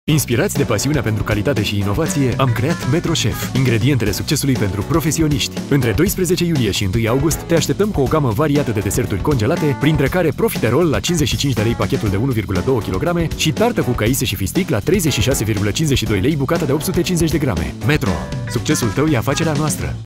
METRO Chef - Profiterol si tarta
Inspirați de pasiunea pentru calitate și inovație am creat METRO Chef. Ingredientele succesului pentru profesionisti.
Între 12 iulie și 1 august te așteptăm cu o gamă variată de deserturi congelate printre care profiterol la  55 lei/ pachetul de 1,2 kg și tartă cu caise și fistic la 36,52 lei/ bucata de 850g.

METRO. Succesul tău e afacerea noastră!